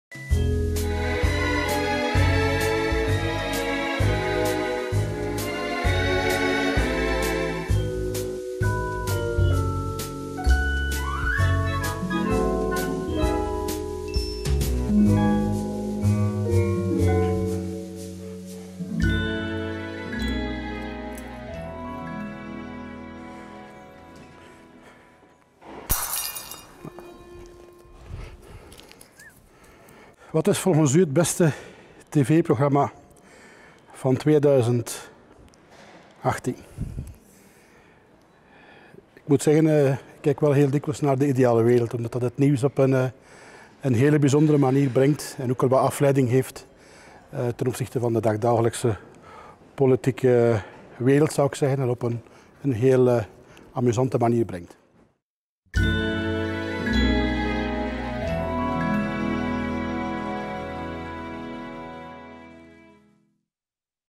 Kerstbalspecial 2018: Guy D'haeseleer
De kerstbalspecial van vlaamsparlement.tv. Dat zijn de klassieke eindejaarsvraagjes maar dan in een breekbare verpakking. Vandaag bepaalt het lot op welke vraag Guy D'haeseleer een antwoord mag formuleren.